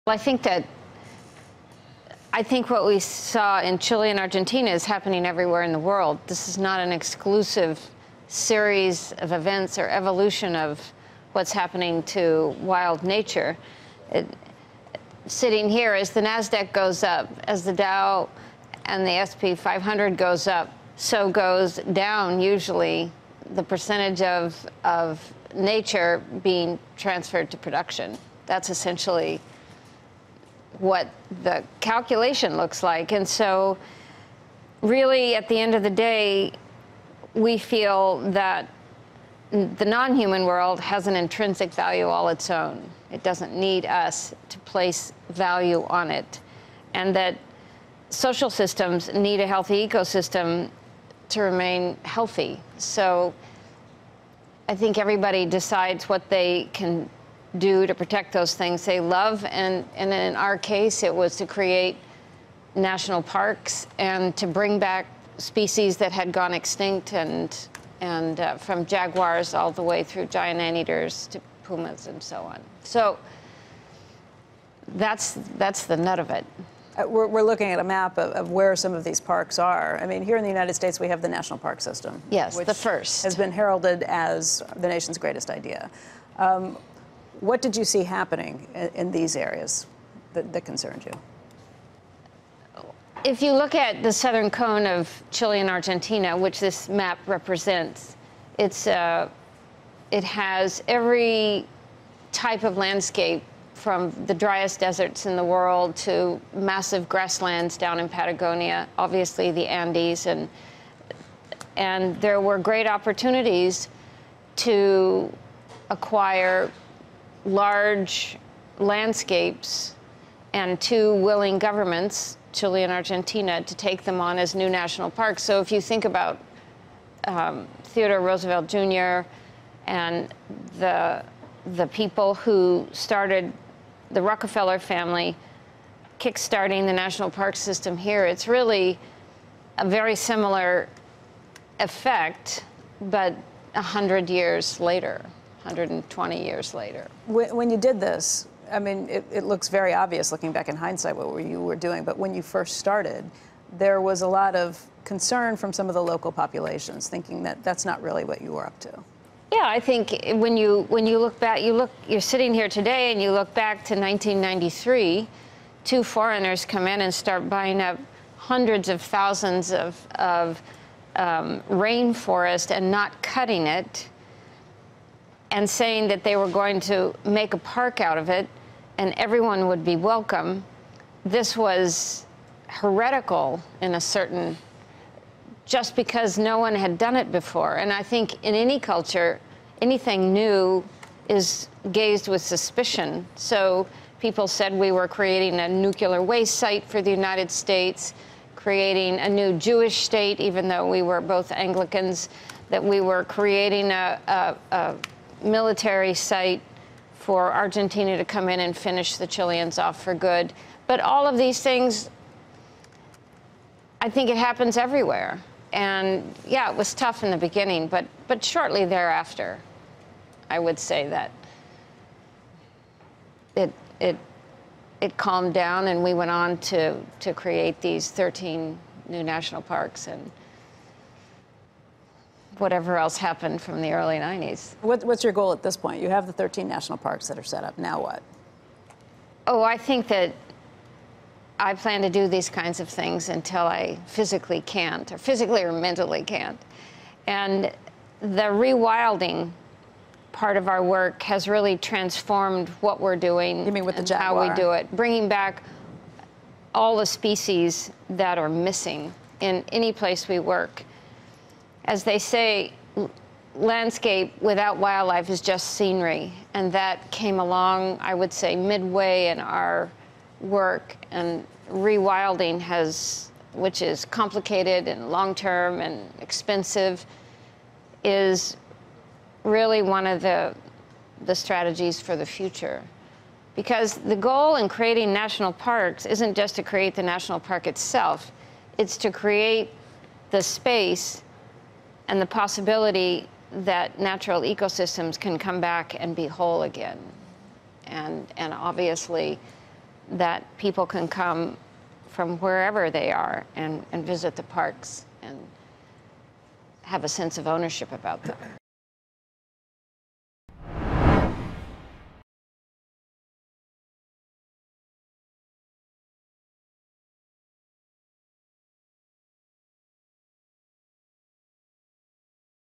Former Patagonia CEO on conservation efforts in South America
Kristine Tompkins, Tompkins Conservation co-founder, president and former Patagonia CEO, discusses her efforts to keep South American parks protected.